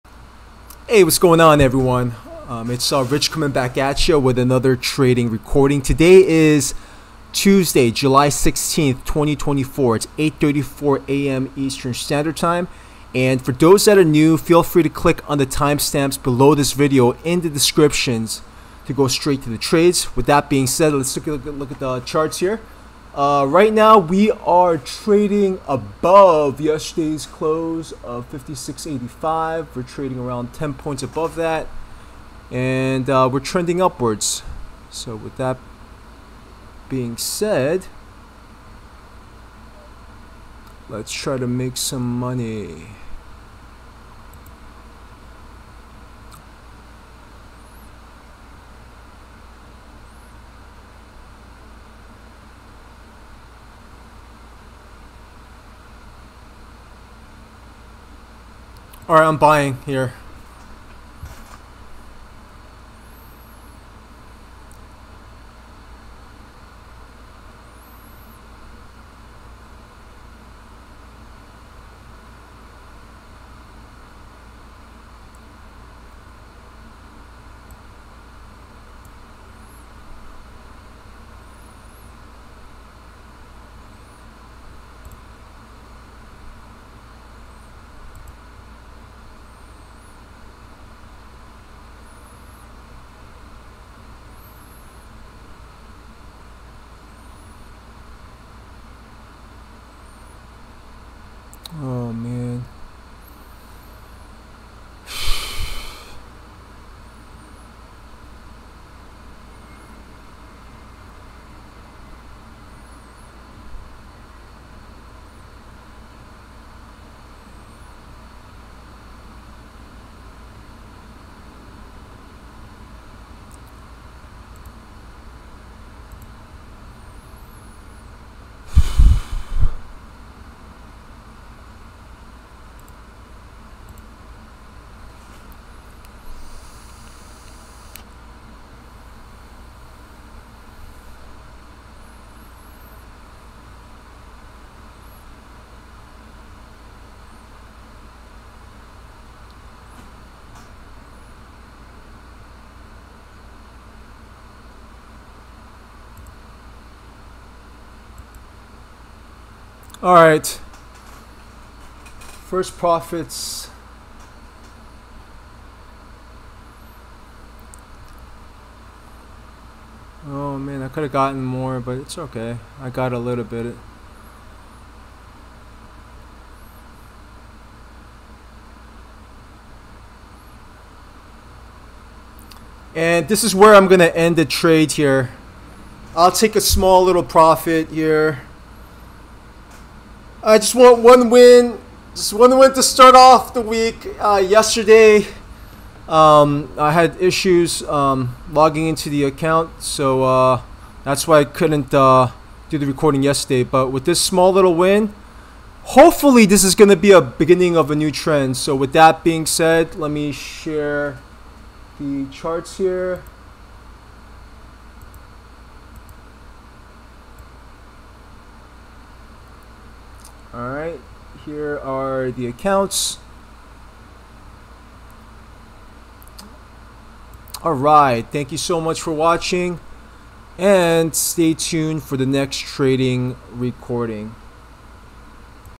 +$1,653.70 Profit 1 trade 7/16/24
0:58 First and only trade (win)
3:41 Showing my evaluation accounts.

1 trade 1 win.

Yesterday, there were issues with the platform so I couldn't trade.  With that being said, for the first and only trade, I went long after a slight pullback and got the win.  Hopefully this trade will be a start to a good week.

Recorded session